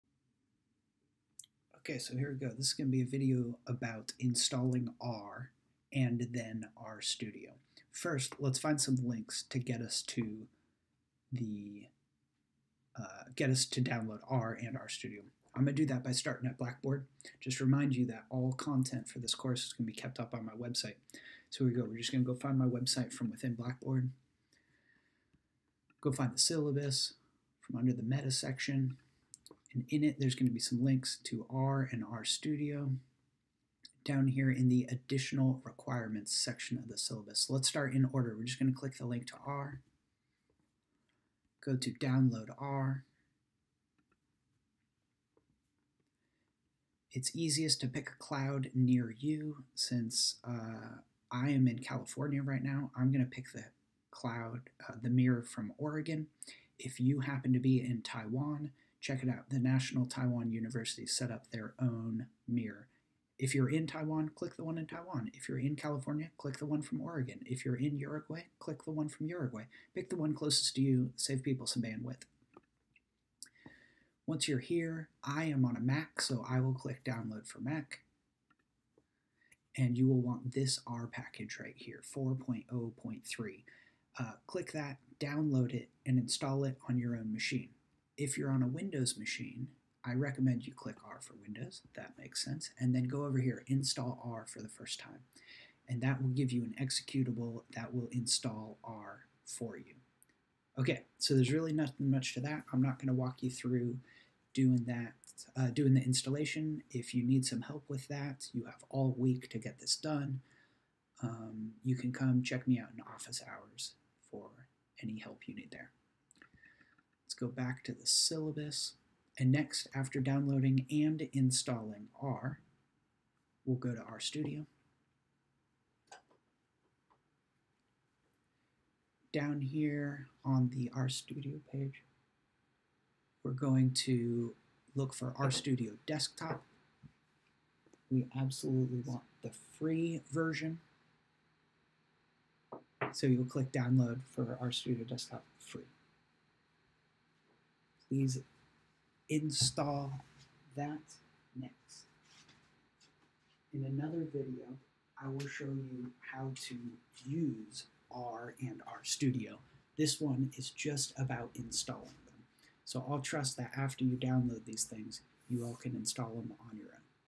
Tutorial for Downloading R and RStudio
A 3 minute video showing the specific links to use for downloading and install R and RStudio